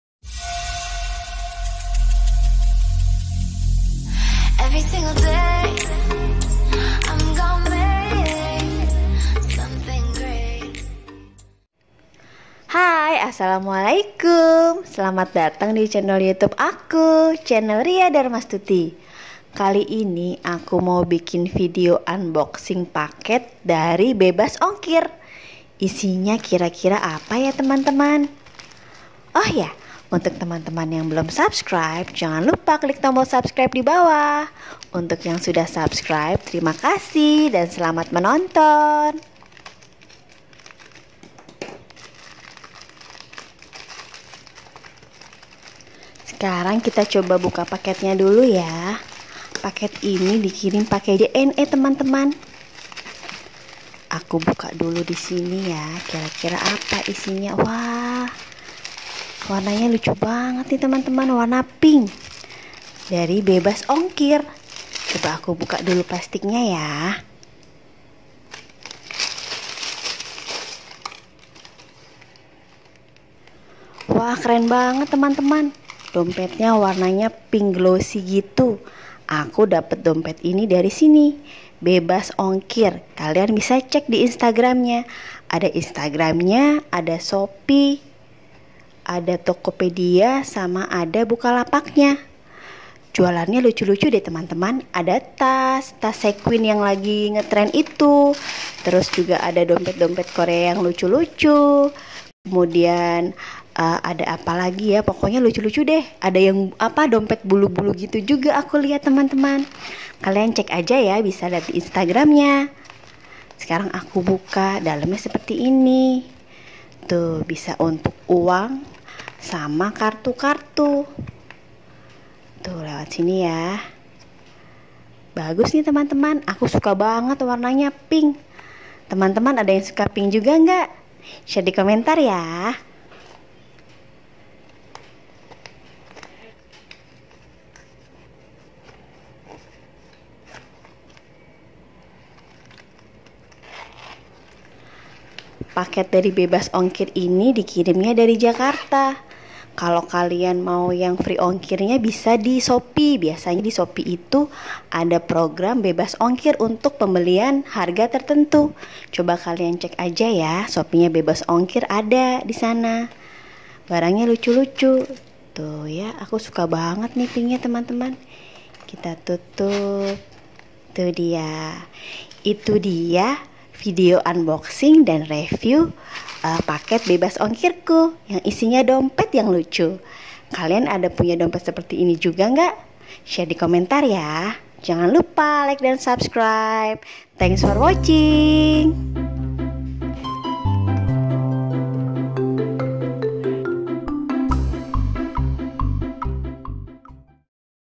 Unboxing Paket dari Bebas Ongkir - Paket Shopee Keren
Hai teman-teman

Kali ini aku bikin video unboxing dan review paket dari online shop BEBAS ONGKIR. Online shop ini ada Instagram, Shopee, Tokopedia dan Buka Lapak nya.

Kira kira isi paket nya apa ya ? Tonton sampai habis ya teman teman 😊

Teman teman paling suka aku bikin video apa? Share di komentar ya.

Follow Instagram ku ya